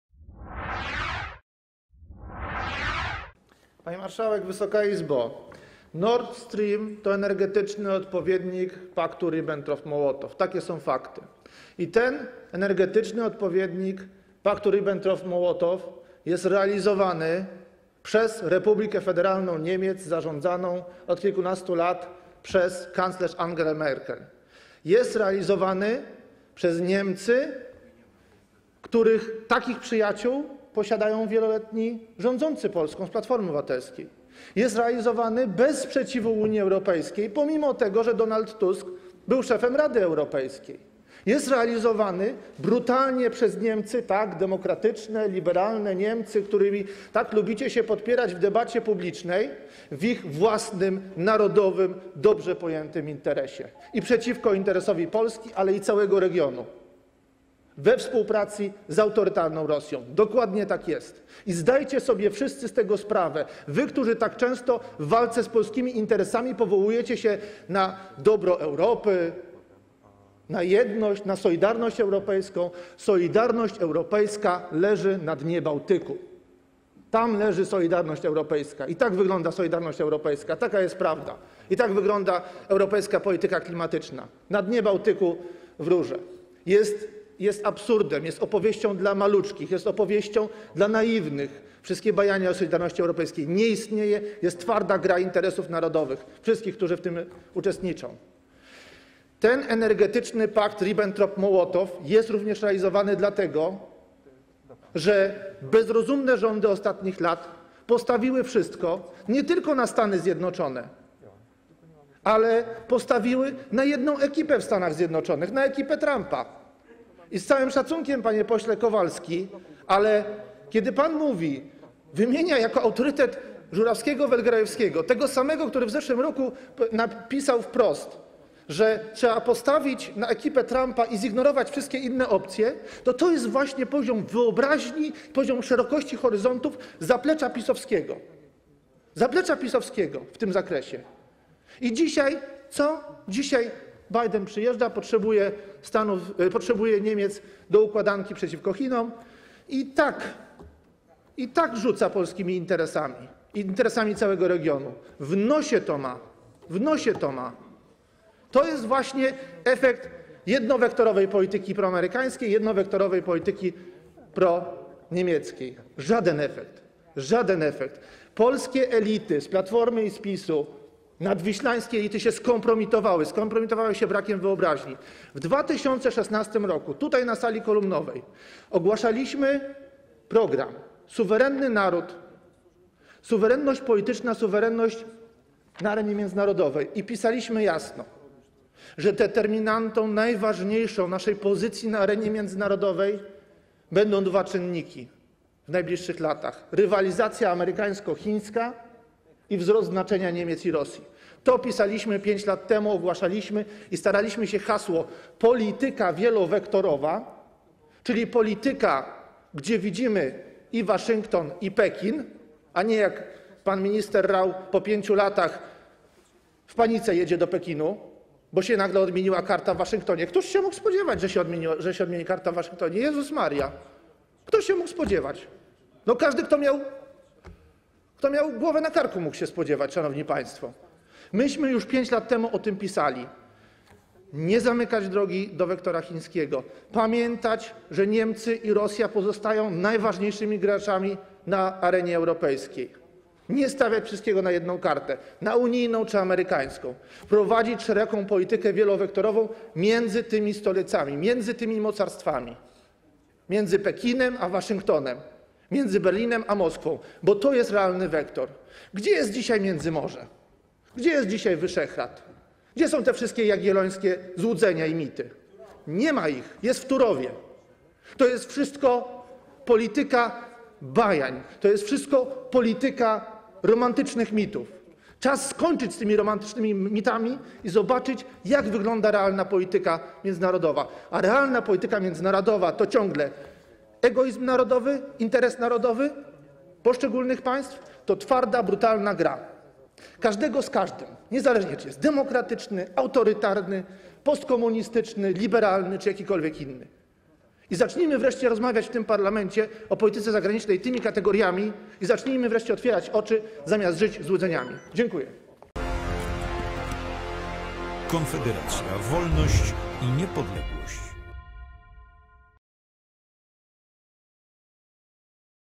Winnicki: Solidarność europejska leży na dnie Bałtyku!
Robert Winnicki o twardej grze interesów narodowych i energetycznym odpowiedniku paktu Ribbentrop-Mołotow.